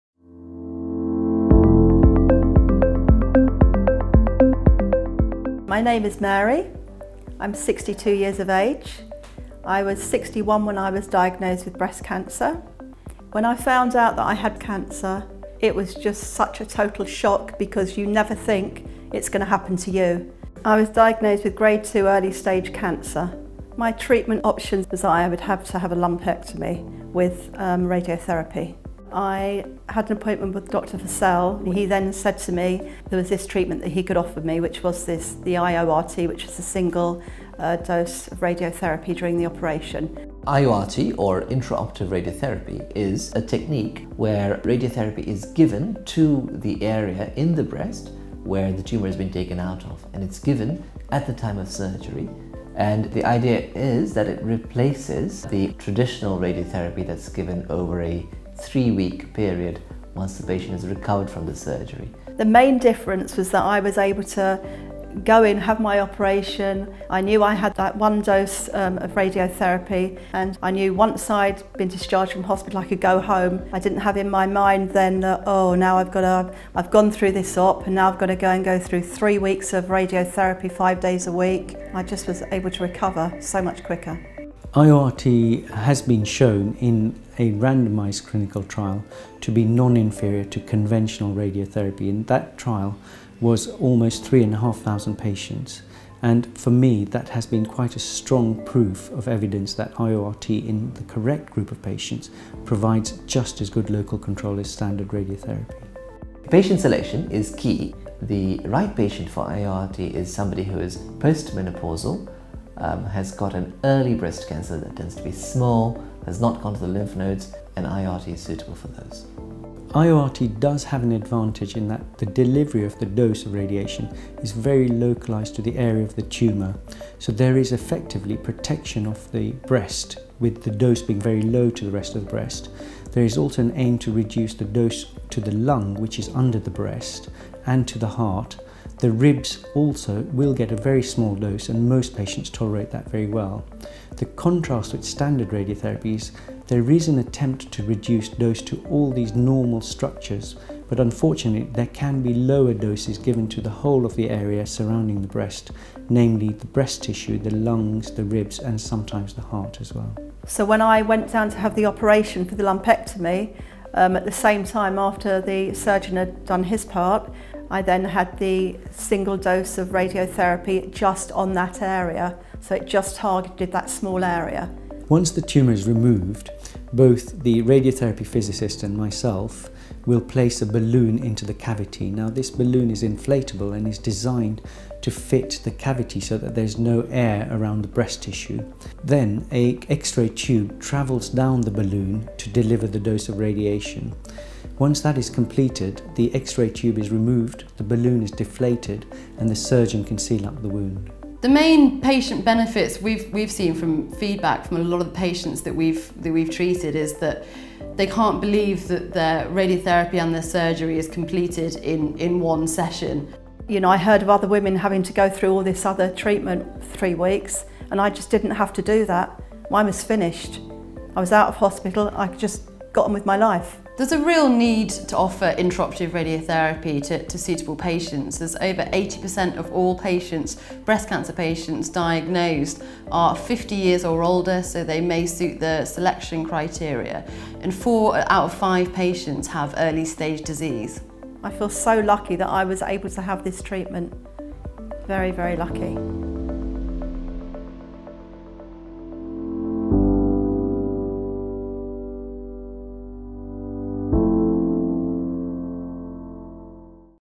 Intraoperative Radiotherapy (IORT) for early stage breast cancer, Giving Patients A Choice
Early stage breast cancer patient, Mary describes her treatment journey with SD-IORT and the difference being offered SD-IORT by her surgeon made to her breast cancer treatment experience. Includes further information about SD-IORT from Consultant Breast Surgeon, Mr Maisam Fazel, Consultant Clinical Oncologist, Dr Nihal Shah and IORT Radiographer Sarah Williams.